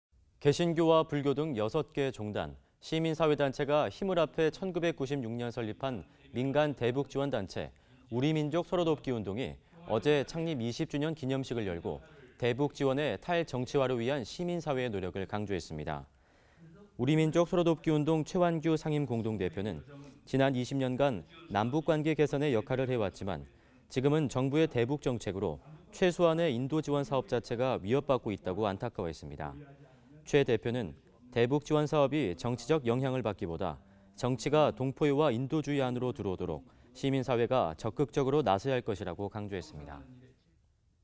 [CBS 뉴스] 우리민족서로돕기 20년
개신교와 불교 등 6개 종단이 시민사회가 힘을 합해
1996년 설립한 민간대북지원 단체, 우리민족서로돕기운동이
어제(지난 21일) 창립 20주년 기념식을 열고
대북지원의 탈 정치화를 위한
시민사회의 노력을 강조했습니다.

우리민족서로돕기운동 최완규 상임공동대표는
지난 20년 간 남북관계 개선에 역할을 해왔지만
지금은 정부의 대북정책으로
최소한의 인도지원사업 자체가
위협받고 있다고 안타까워했습니다.

최 대표는
대북지원 사업이 정치적 영향을 받기 보다
정치가 동포애와 인도주의 안으로 들어오도록
시민사회가 적극적으로 나서야 할 것이라고 강조했습니다.